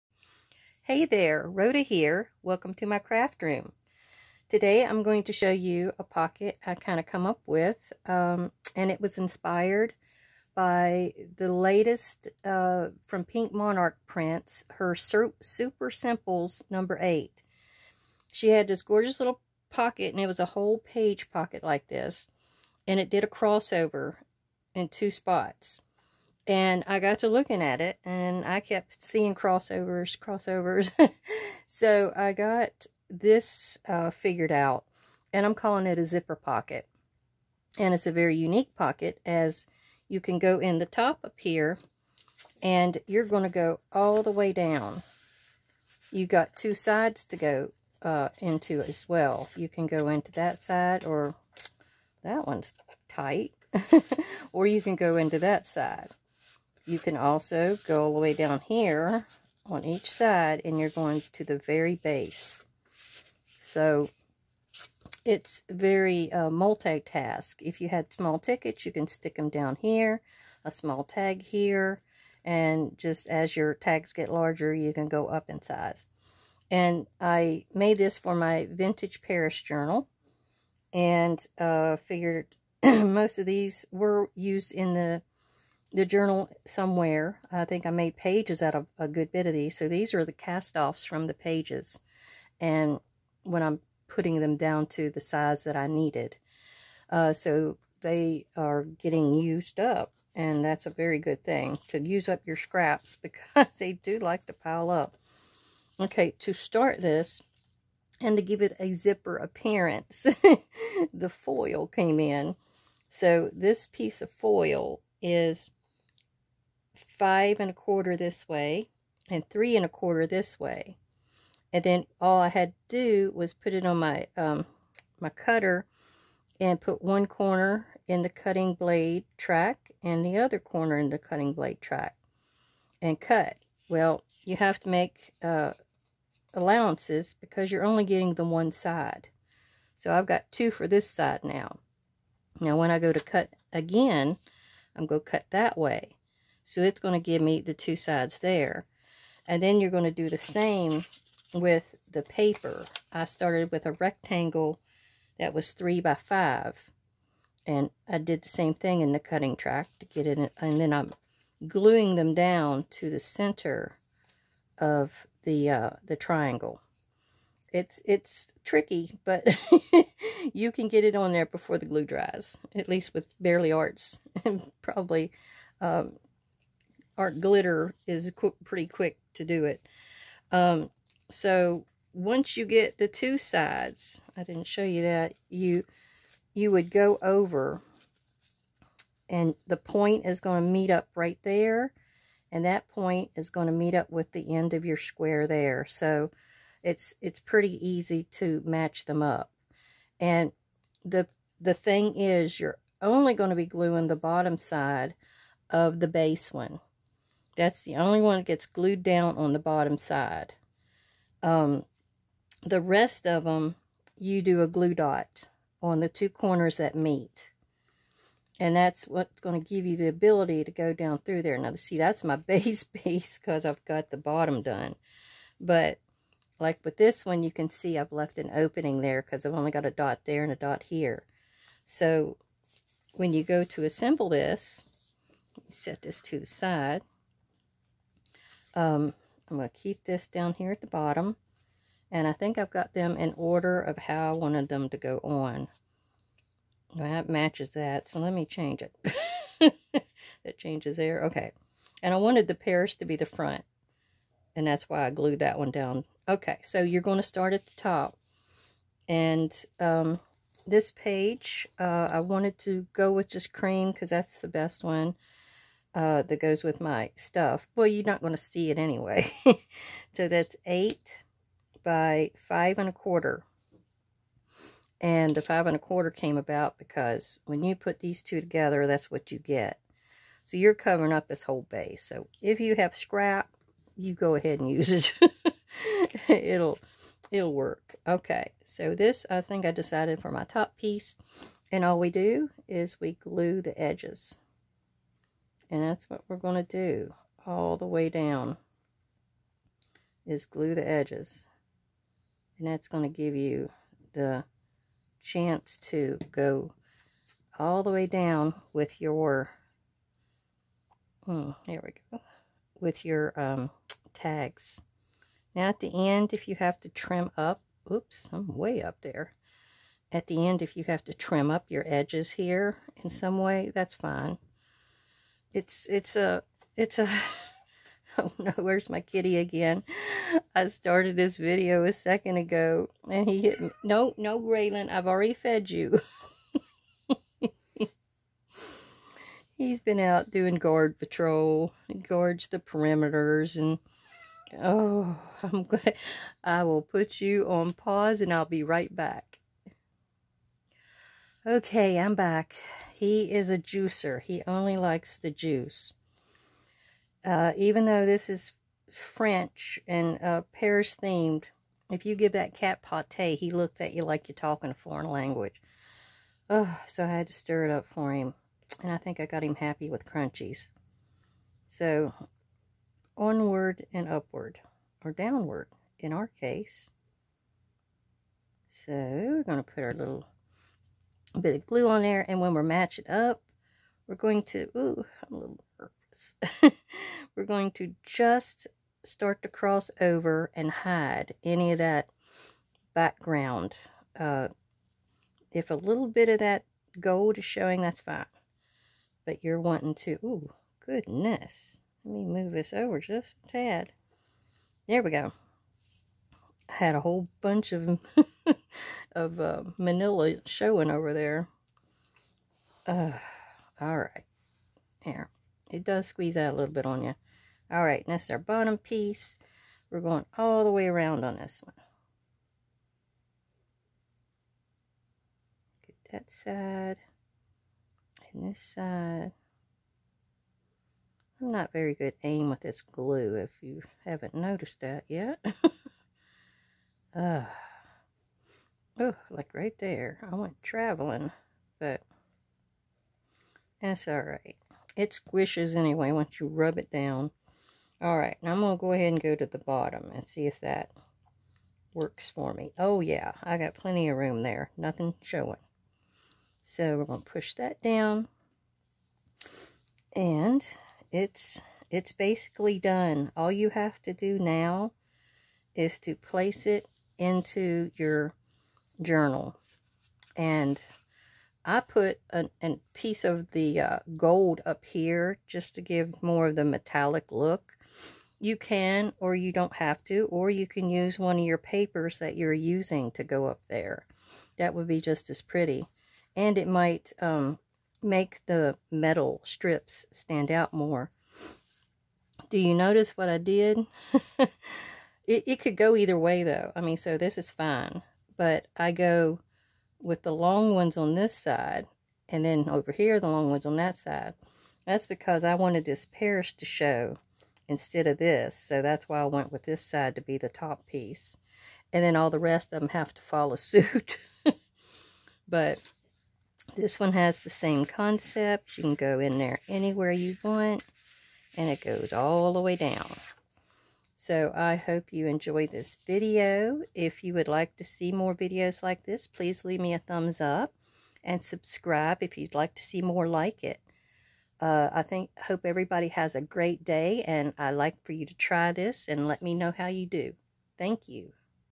Easy multi Zipper Pocket for junk journals and more!
Hey there, Rhoda here!  Welcome to my craft room!
My ideas come to me in many ways, but they usually revolve around me seeing something in a video, flip thru or Instagram, and my mind expanding on the idea in some way.    This idea came from seeing a cross over pocket and I 'saw' it being like a corset, but my hands actually formed it into a zipper.    Hope you enjoy!

Come join us!  Here I share some of my day-to-day going-ons in the journal world.